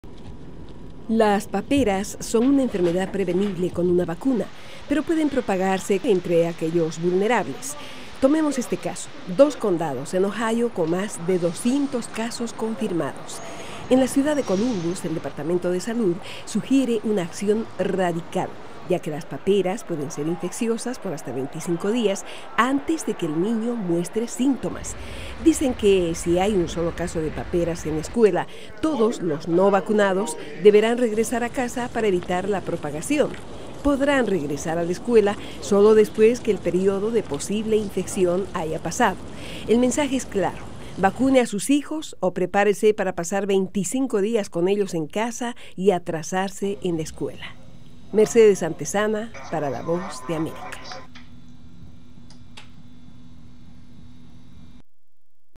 Incremento en casos de paperas
Es una enfermedad que probablemente ni usted ni su médico haya visto. Y no es exótica ni tropical, pero mantendrá a su hijo en casa hasta por tres semanas.